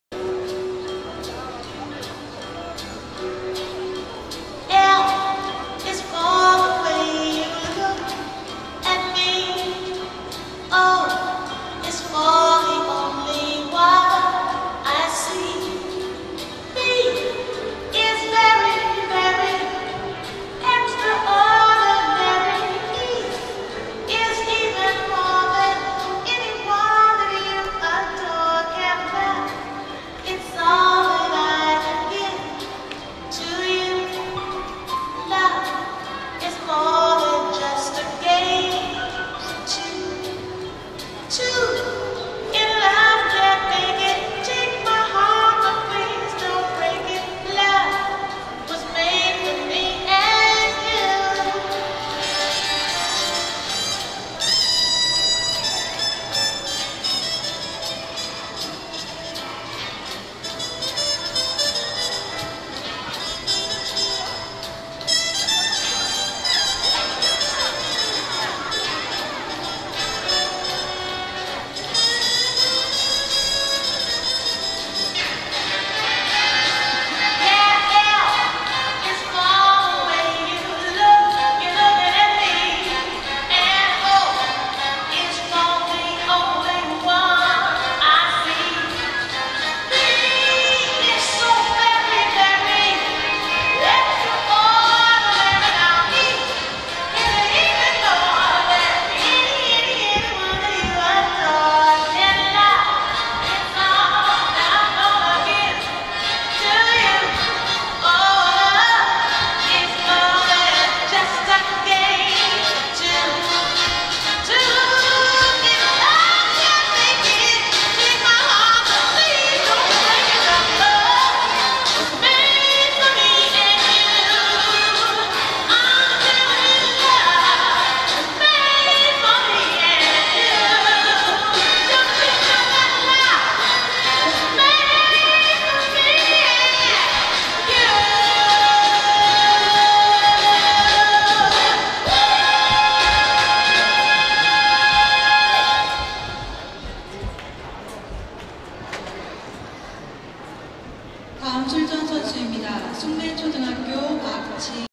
여자2급 FS 4학년1조 김태연,광정초4 @ 2017년 교보생명컵 피겨 꿈나무대회
2017/08/04, 전주 화산 실내빙상장, 2017년 제33회 교보생명컵 피겨스케이팅 꿈나무 체육대회